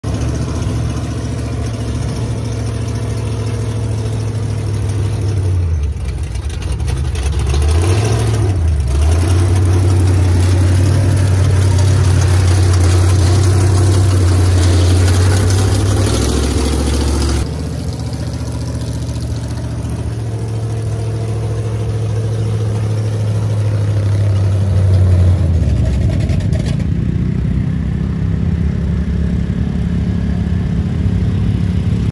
Eight Reasons Swamp Buggy can Float
Wait, yes, there are eight huge flotation tractor tires on the swamp buggy. This driver is about to go compete in the floating island competition here at Plantbamboo in Okeechobee Florida. Be sure to check out the full length video and watch him cross the lake.